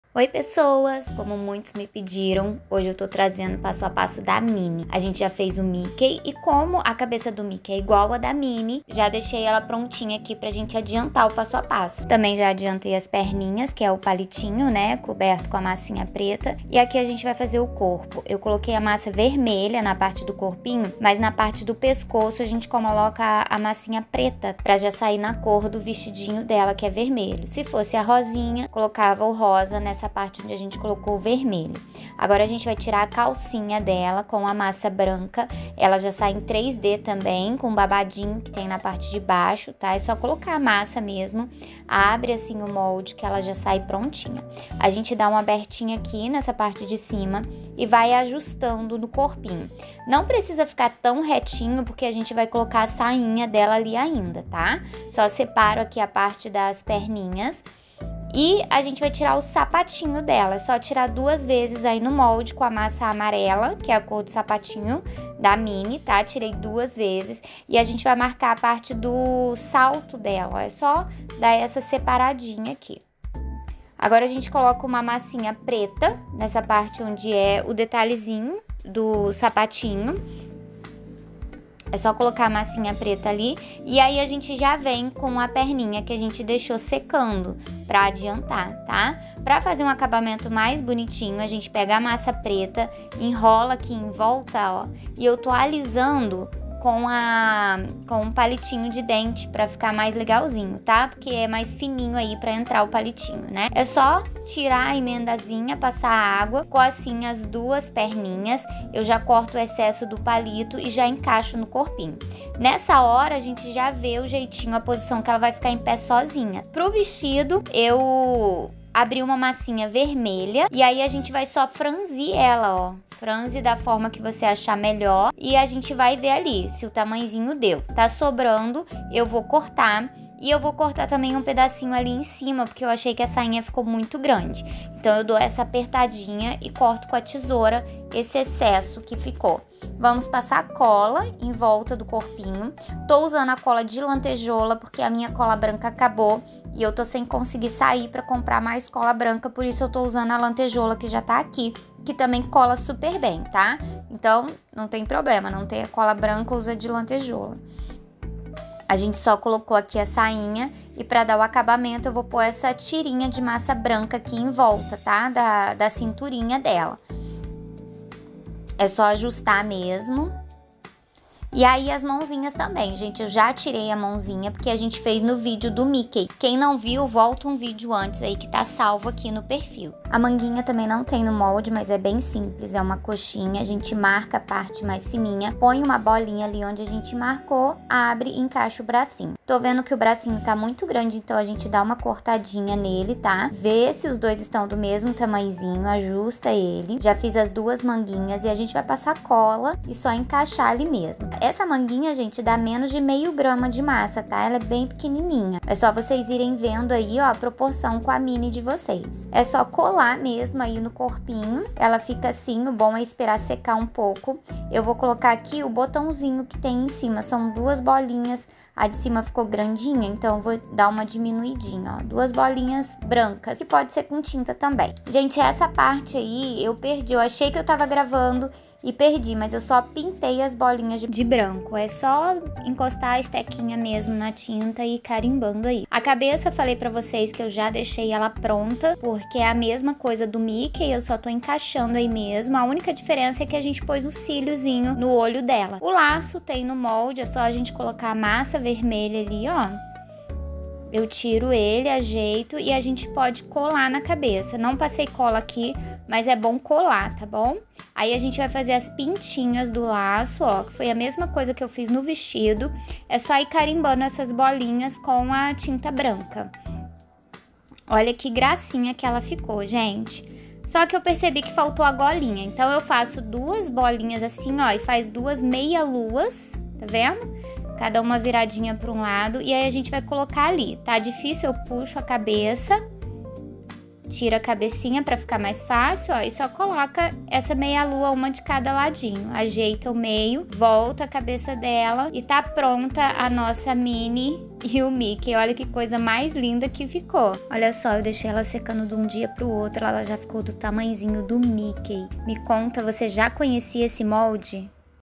Minnie para lembrancinhas - passo a passo
A Minnie fica com cerca de 9cm depois de seca.

Molde utilizado: Molde Ratinho e Ratinha - Moldes da Rê